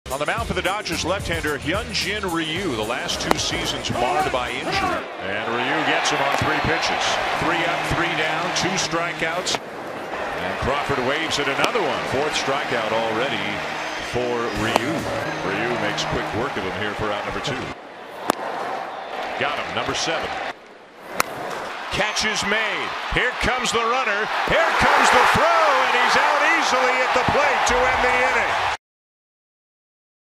SF@LAD: Ryu fans seven in seven scoreless innings
Hyun-Jin Ryu strikes out seven over seven scoreless innings, allowing five hits and one walk vs. the Giants